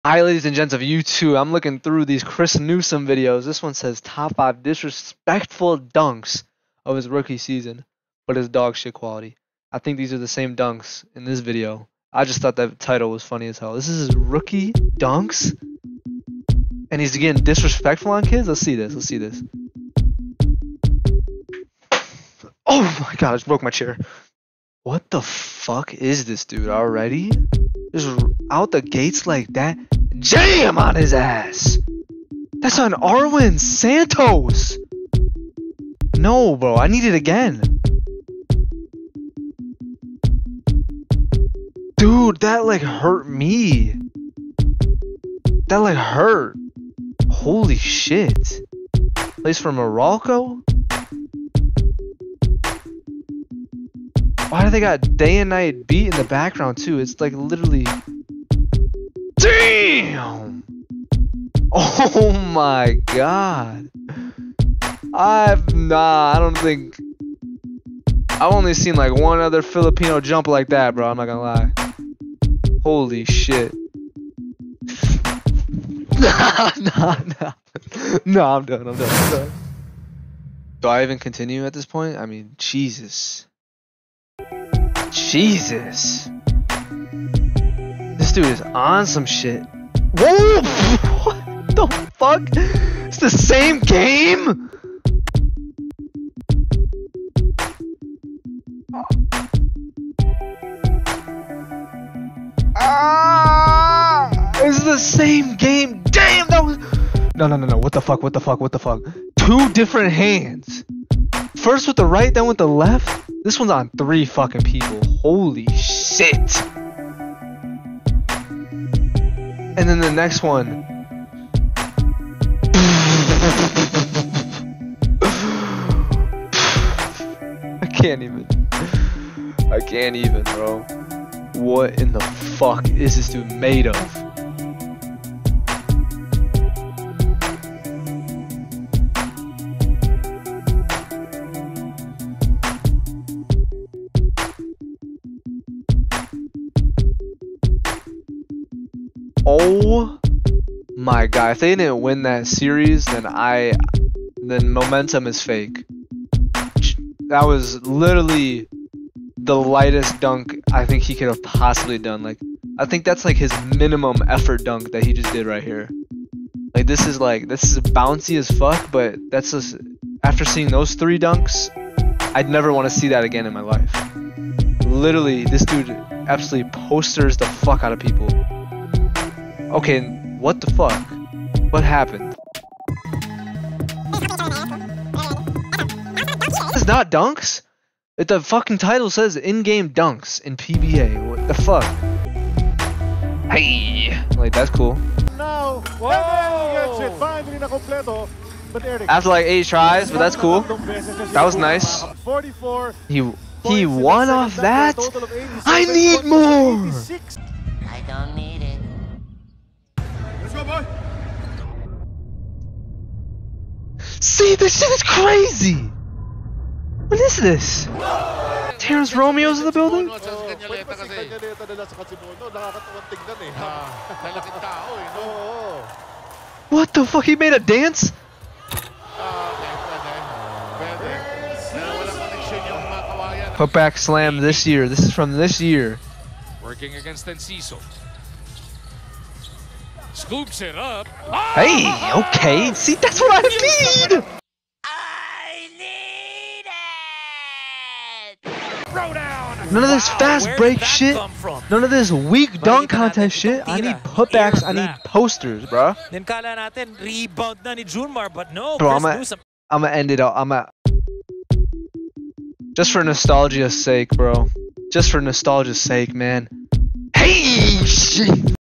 CHRIS NEWSOME IS A FREAK OF NATURE! TOP DUNK HIGHLIGHTS
this dude chris newsome is JUMPIN JUMPIN... like out the gym no kizzy
chris newsome top dunk highlights of his career!

road to 1 thousa

Matthew insta: matthewpoulos15
Chris Newsome
chris newsome highlights
dunk pba
pba highlights
philippines basketball
chris newsome dunk
fiba